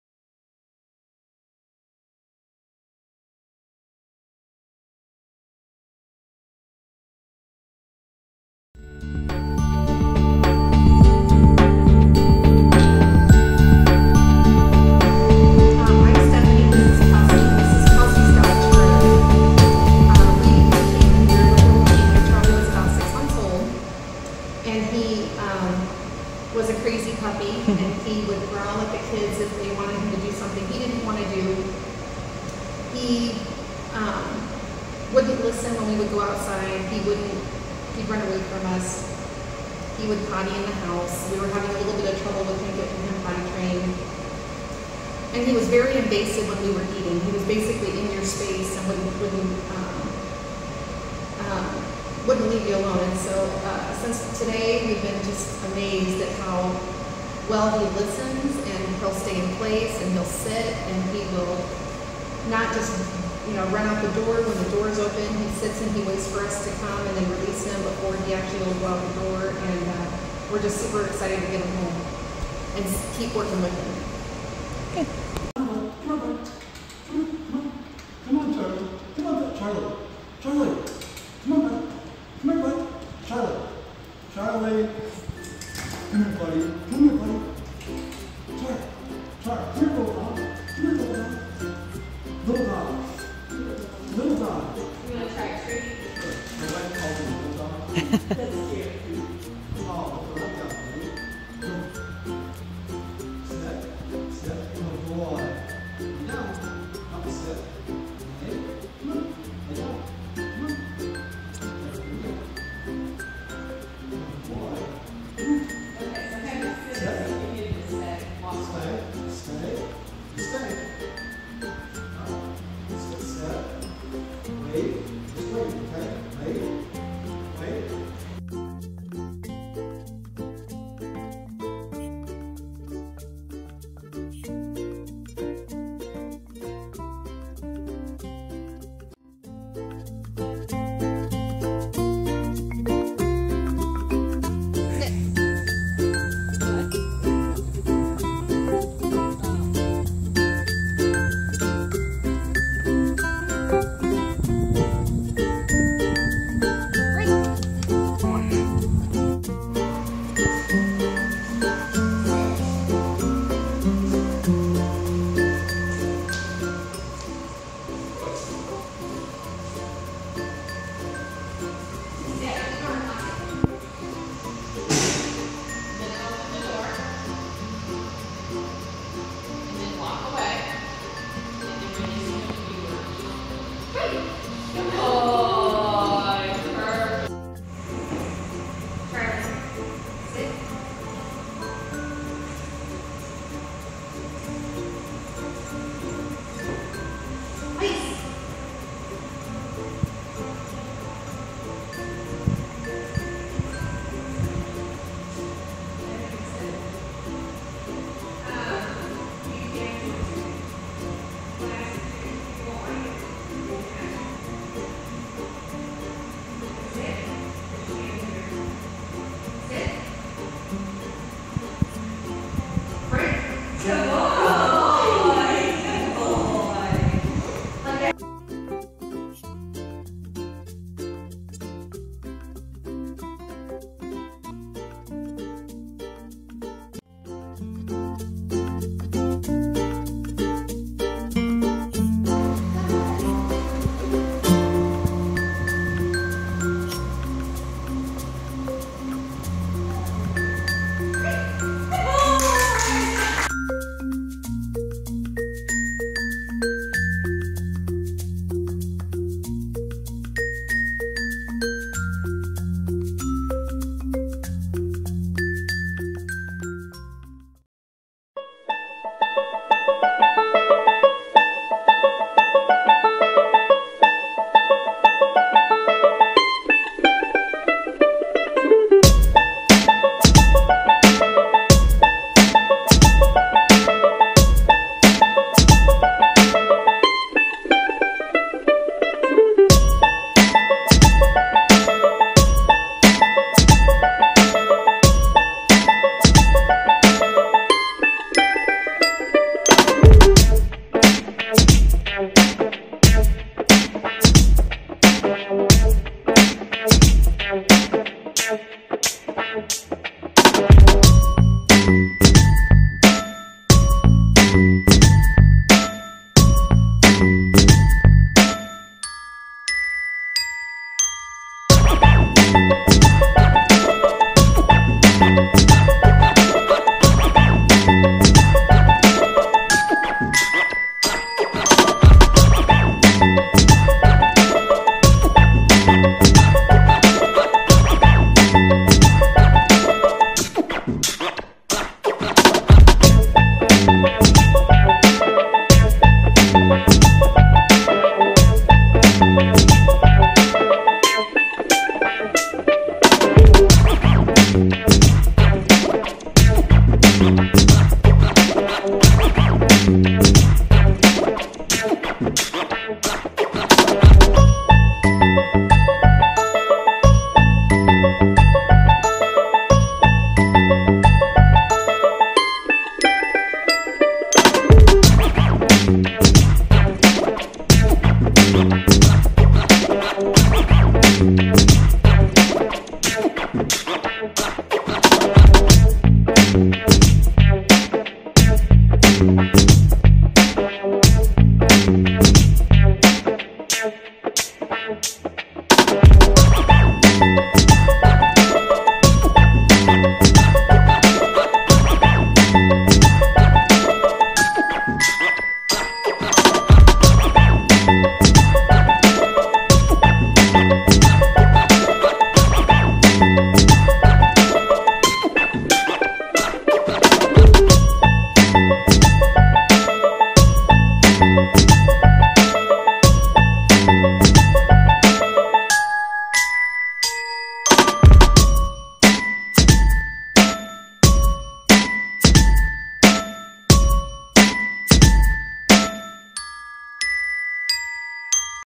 Charlie - 6mo old Shihpoo Omaha Nebraska Dog Training, 2 week Board & Train Program
2 week Board & Train
Basic Obedience
bite work
nose work
scent training
k9 training
Canine training
Dog trainers
dog training
dog work